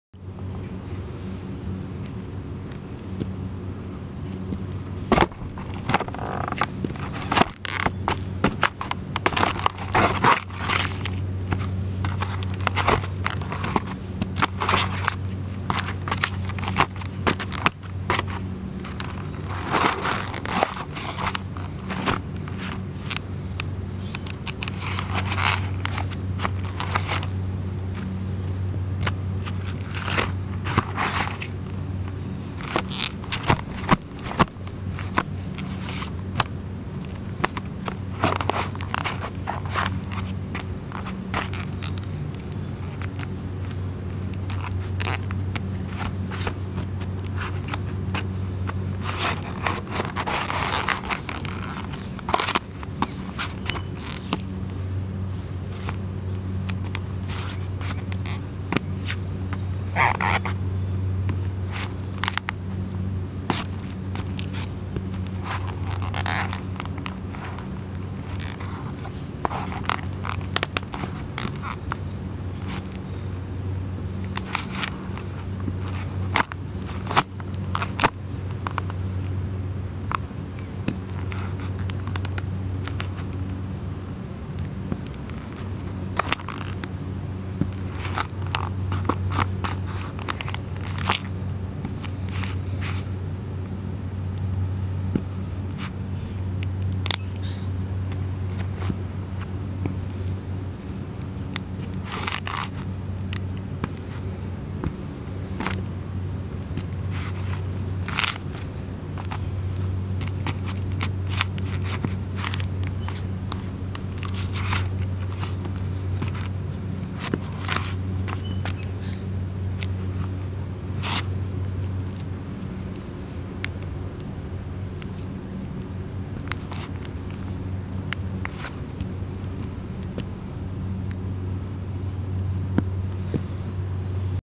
Herman Survivors vintage work boot crunch on a pizza box
My Herman Survivors vintage work boots stomp and crush an empty pizza box